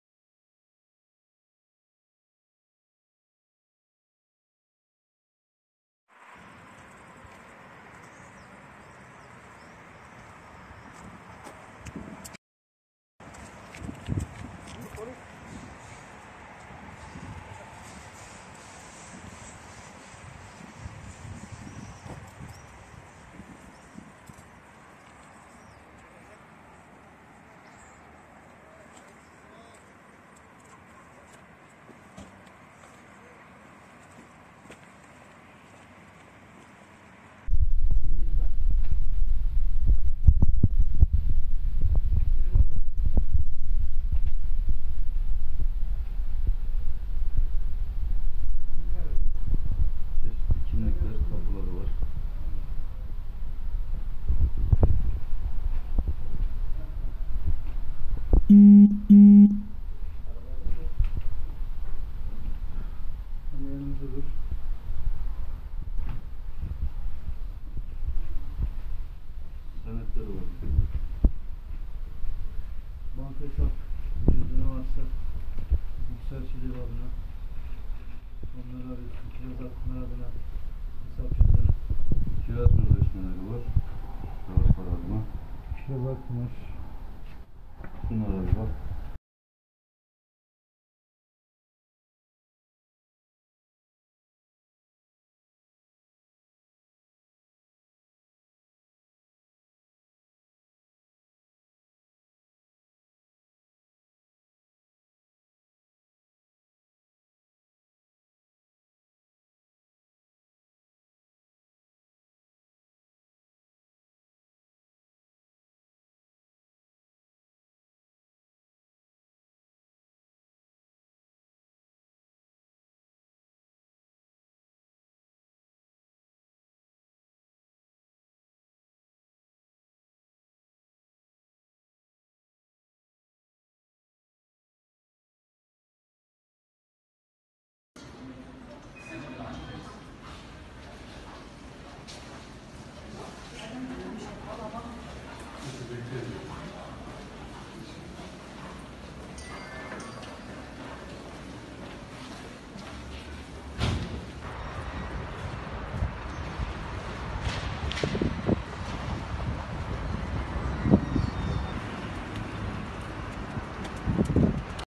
BANKER BİLO GERÇEK OLDU / Yurt dışına işçi götürme vaadiyle dolandırıcılık yapan çete çökertildi
Dolandırıcılar, Avrupa’ya gönderme vaadiyle paralarını aldıkları mültecileri, Kayseri’ye giden bir TIR’a bindirdi. Mola tesisinde durumu fark eden mülteciler, dorsenin çadırını kesip kaçtı.